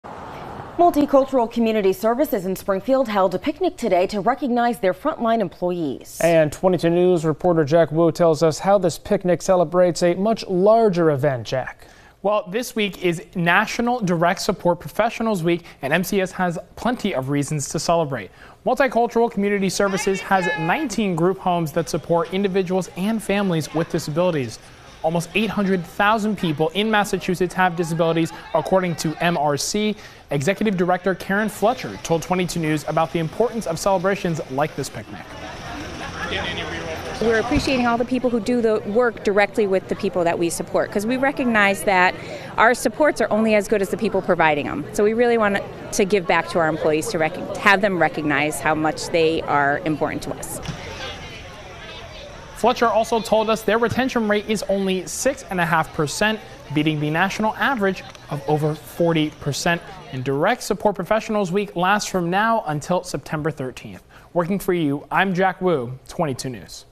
Multicultural Community Services in Springfield celebrates their frontline workers
Multicultural Community Services in Springfield held a picnic Wednesday to recognize their front-line employees as part of National Direct Support Professionals Week.